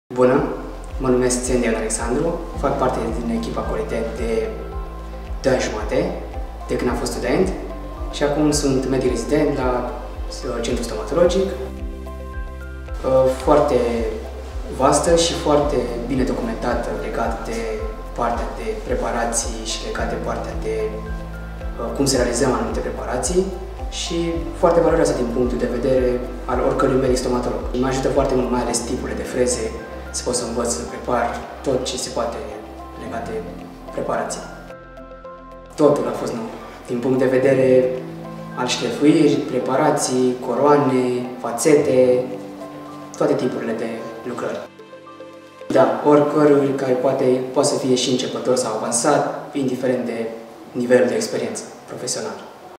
Testimonial participant curs Cerec MasterClass
Afla si tu ce parere au participantii cursului Cerec MasterClass modulul 1, daca au aflat lucruri noi si ce vor pune in practica din prima zi in cabinet